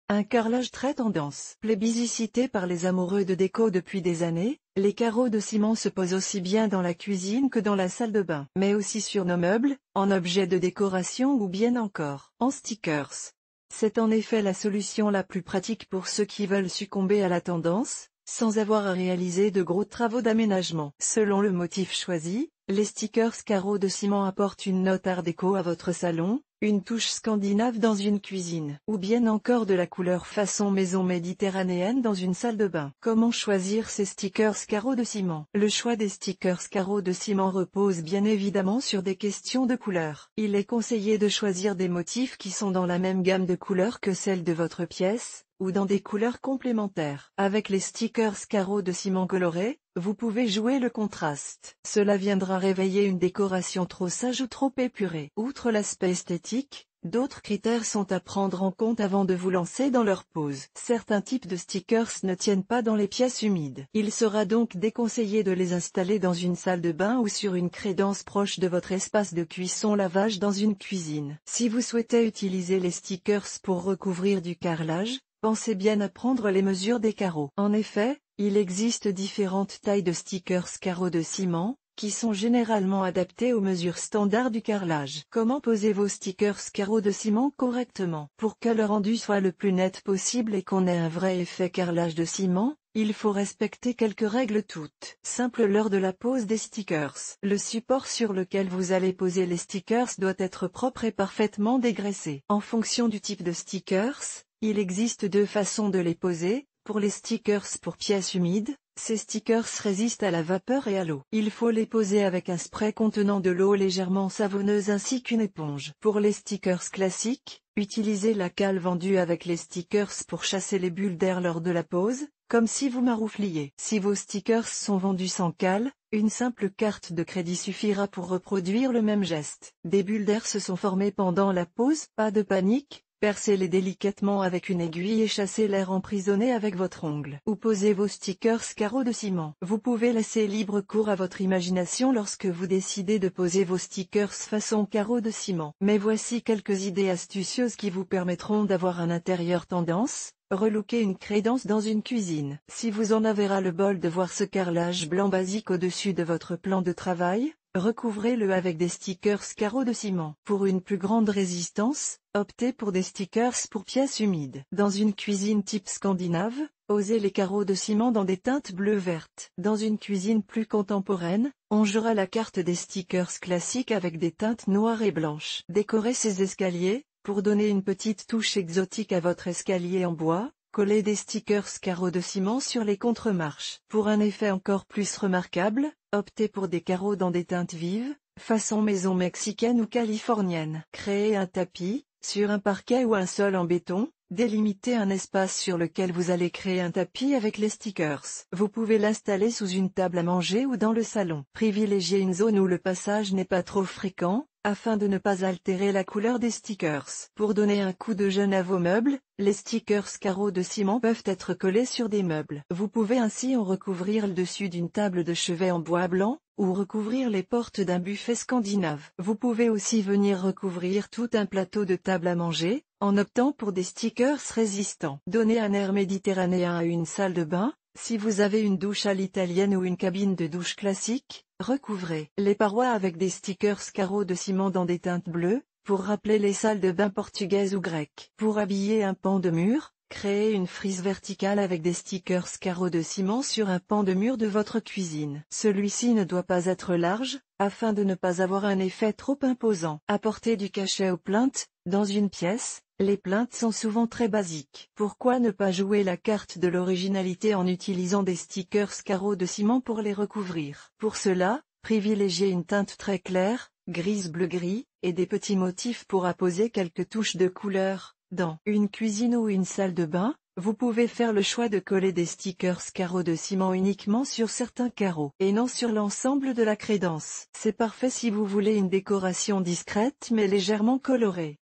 8 idées déco pour décliner les carreaux de ciment !
Comment adopter les stickers carreau de ciment dans sa déco

Les carreaux de ciment, on ne s'en lasse pas ! Ils apportent du cachet à chaque pièce de la maison. Souvent disposés sur les sols et les murs, ils se déclinent également sur les meubles et les objets déco, de la table basse aux stickers muraux. Découvrez vite comment céder à la tendance, grâce aux